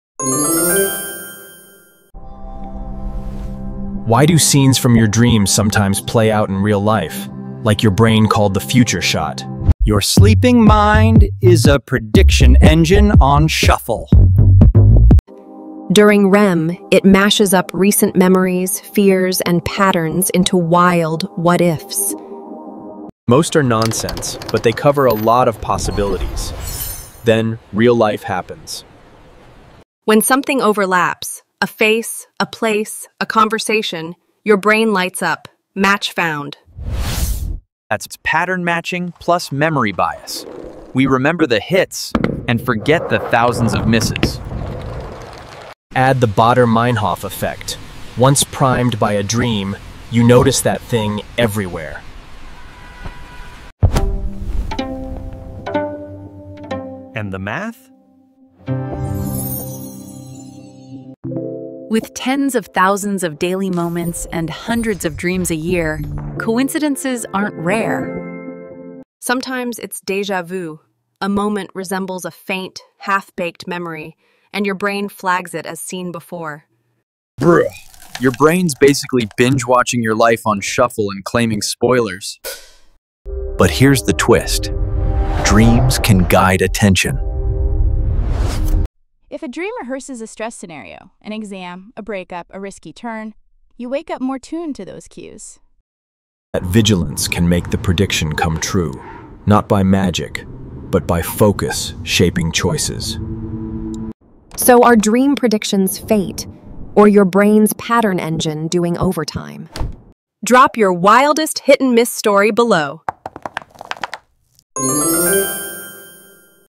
Why Do Dreams Sometimes Come True? Déjà Vu Explained
Dreams feel prophetic? Here’s the psychology behind déjà vu and “predictions.”
During REM, your brain runs future sims; confirmation bias, Baader–Meinhof, and priming make some scenes play out IRL. Not magic—modeling.
For more brain & behavior breakdowns, hit the channel.